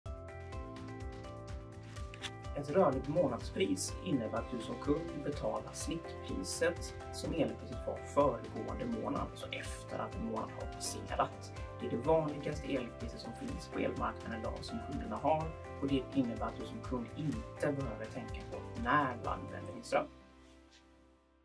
Vad är rörligt elpris?
Vår kundservice svarar på några av de vanligaste frågorna vi får in. En av dem är "Vad är rörligt månadspris?"
Här kan du läsa vad som skiljer rörligt månadspris och rörligt timpris